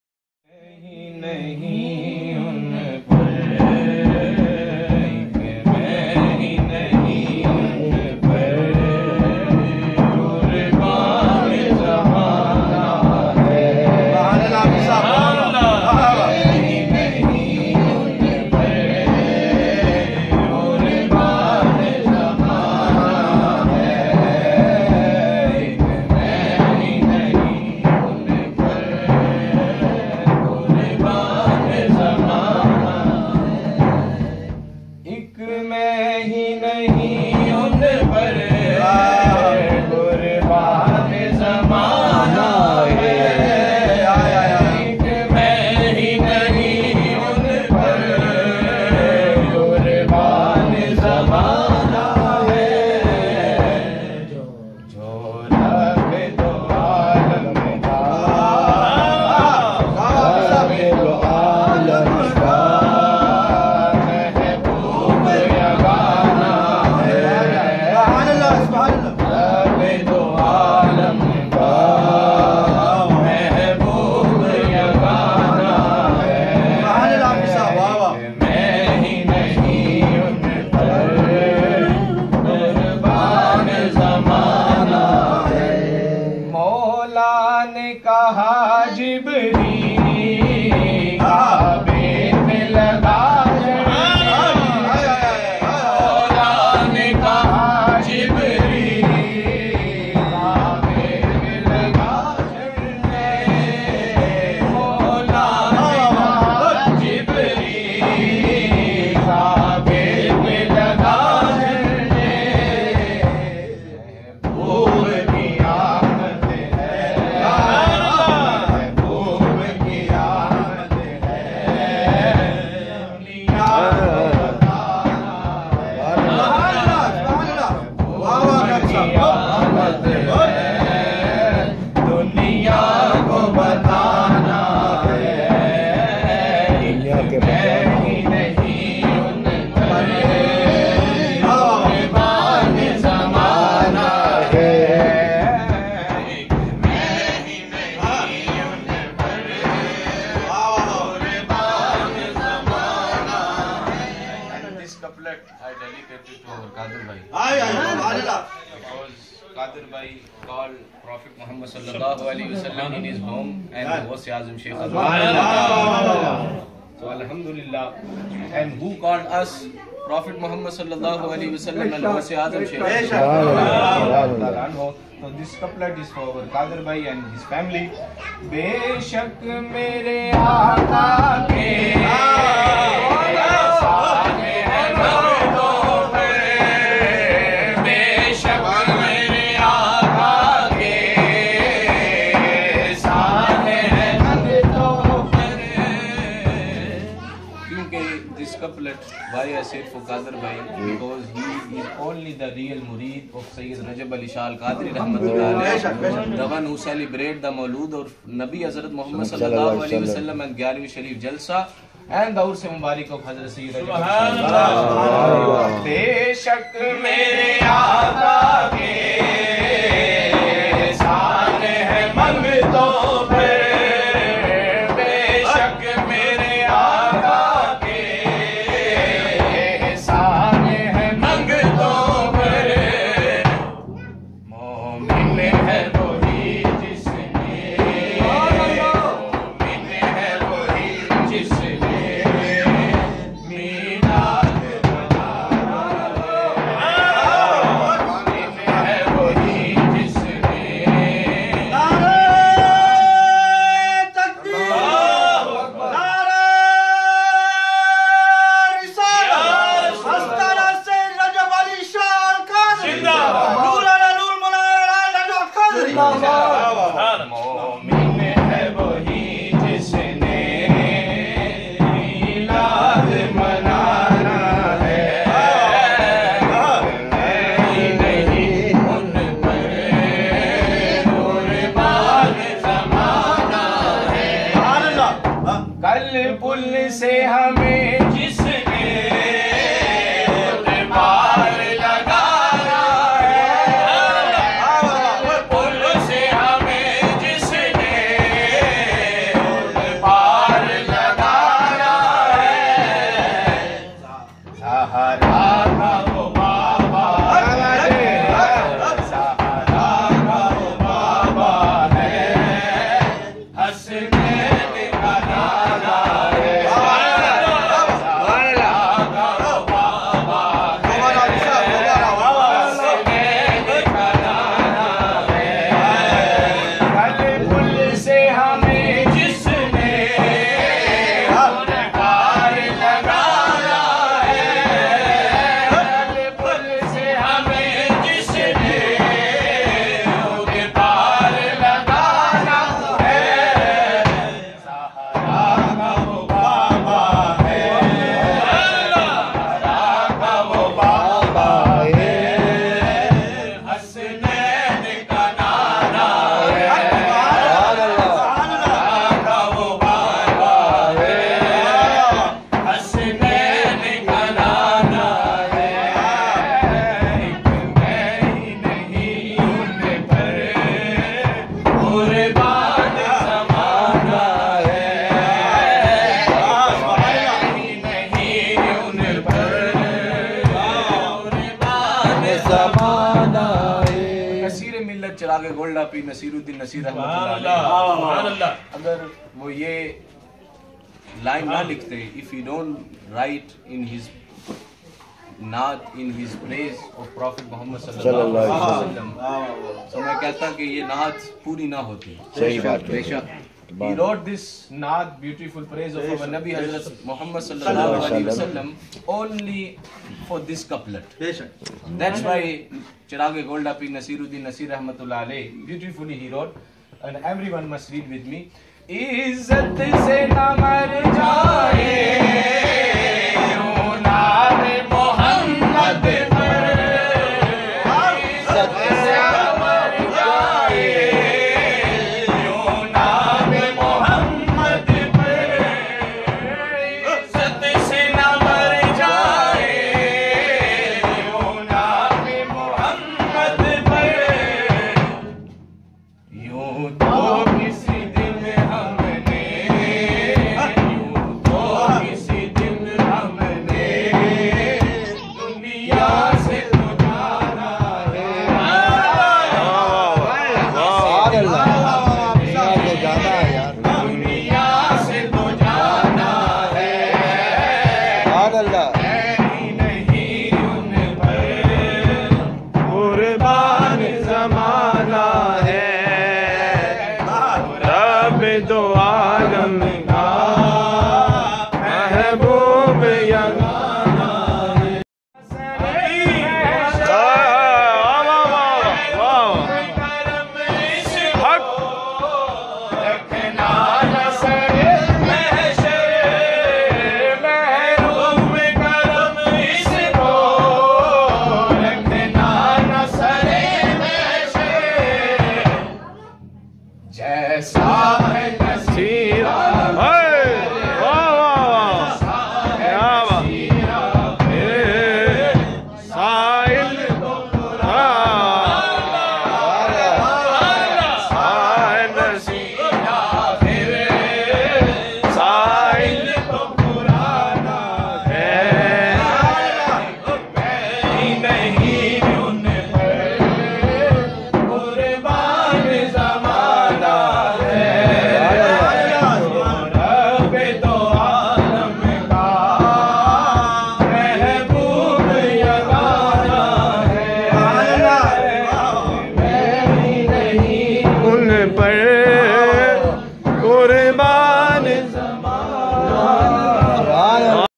"Shauban-ul-Moazam 1437 Special"
Naathey Rasooley Maqbuul ﷺ By: "Sahibzada Hafiz Raja Hamza Khan Al-Qadri Sahib" In First Mubarik Evening Of The 2 Days Grand 15 Annual Jashney Eid Meeladun Nabi ﷺ, Egarwie Sharieef & Urs-e-Mubarik Jalsa Of "Hazrath Peer Sayed Rajab Ali Shah Al-Qadri(R.A)" with Esaley Sawab Mehfil For All Marhooms, Event was Hosted By: "Mr & Mrs Haji Abdul Kader Amod & Family", Saturday 14th May 2016 (After Salat-ul-Esha)